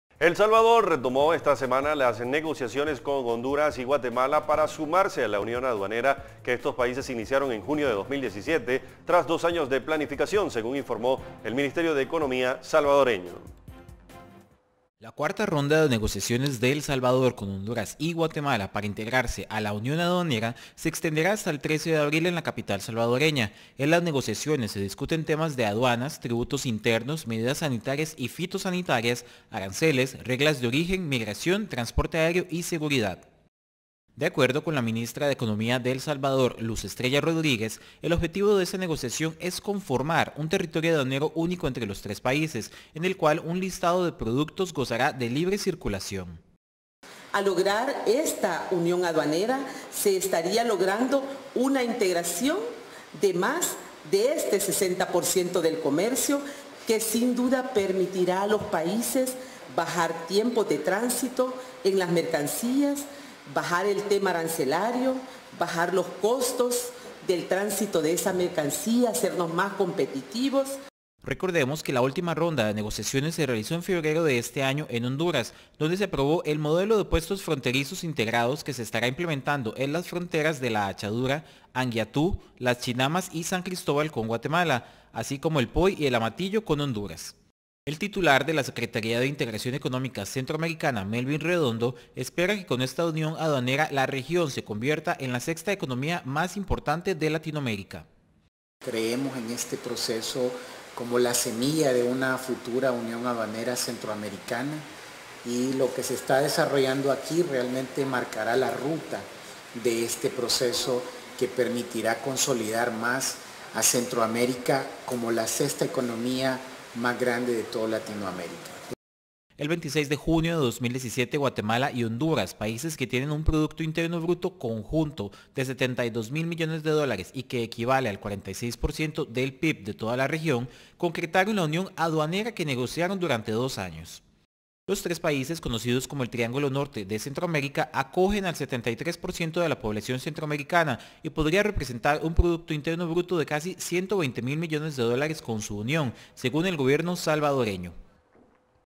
El Salvador retoma negociaciones para integrase a unión aduanera con Guatemala y Honduras
El Salvador retomó esta semana las negociaciones con Honduras y Guatemala para sumarse a la unión aduanera que estos países iniciaron en junio de 2017 tras dos años de planificación, según informó el Ministerio de Economía salvadoreño.

La cuarta ronda de negociaciones de El Salvador con Honduras y Guatemala para integrarse a la unión aduanera se extenderá hasta el 13 de abril en la capital salvadoreña, en las negociaciones se discuten temas de "aduanas, tributos internos, medidas sanitarias y fitosanitarias, aranceles, reglas de origen, migración, transporte aéreo y seguridad".

De acuerdo con la ministra de Economía de El Salvador, Luz Estrella Rodríguez, el objetivo de esta negociación es conformar un territorio aduanero único entre los tres países, en el cual un listado de productos gozará de libre circulación.

La última ronda de negociaciones se realizó en febrero de este año en Honduras, donde se aprobó "el modelo de puestos fronterizos integrados que se estará implementando en las fronteras de La Hachadura, Anguiatú, Las Chinamas y San Cristóbal con Guatemala, así como en El Poy y El Amatillo con Honduras".

El titular de la Secretaría de Integración Económica Centroamericana, Melvin Redondo, espera que con esta unión aduanera la región se convierta en la sexta economía más importante de Latinoamérica.

El 26 de junio de 2017, Guatemala y Honduras, países que tienen un PIB conjunto de $72.781 millones y que equivale al 46% del PIB de toda la región, concretaron la unión aduanera que negociaron durante dos años.

Los tres países, conocidos como el Triángulo Norte de Centroamérica, acogen al 73 por ciento de la población centroamericana y "podría representar un Producto Interno Bruto de casi $120.000 millones" con su unión, según el Gobierno salvadoreño.

Redacción / CB24